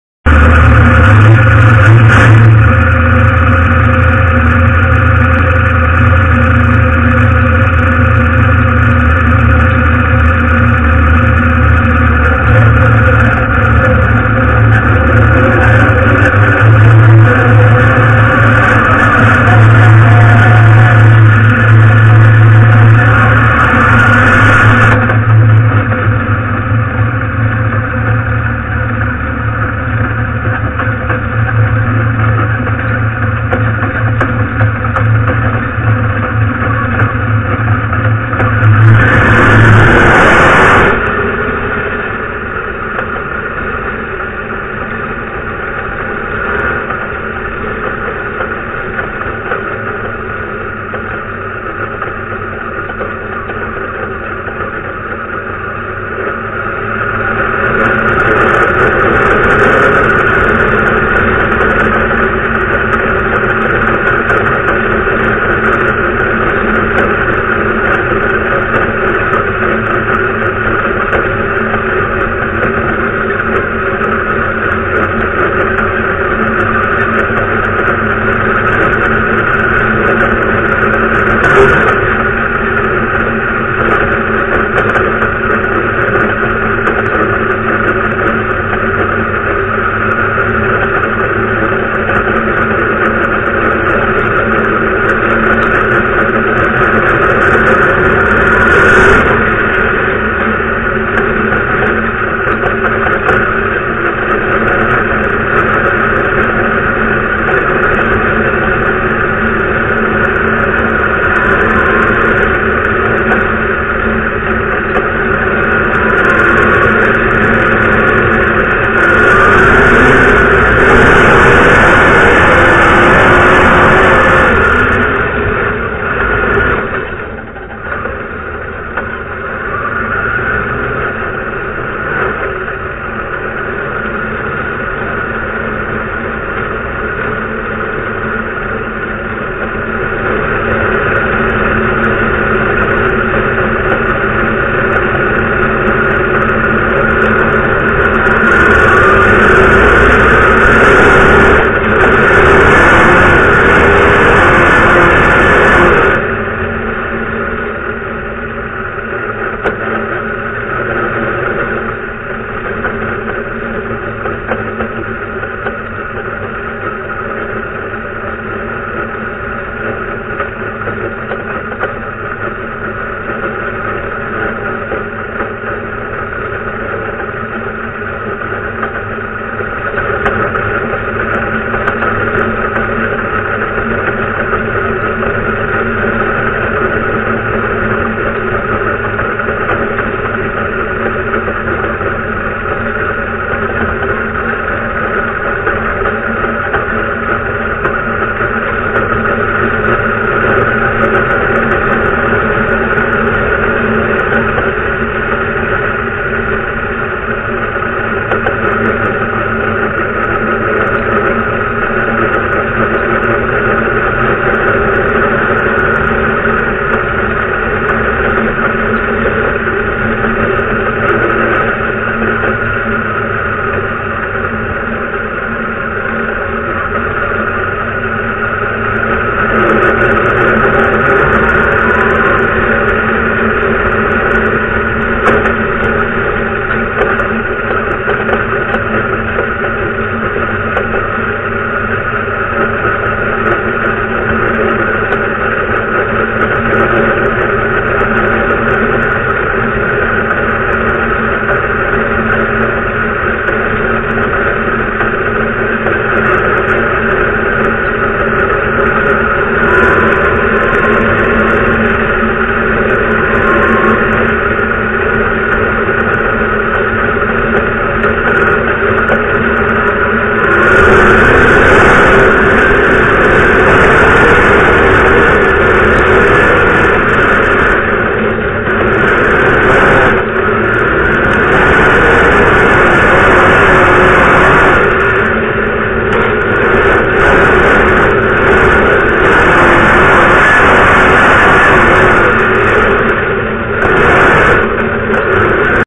Inside car cam Keith Bills Emod
Emod heat race at 35 Raceway on June 7 2014 with Keith Bills of Hurricane West Virginia this video was taken inside the 36k machine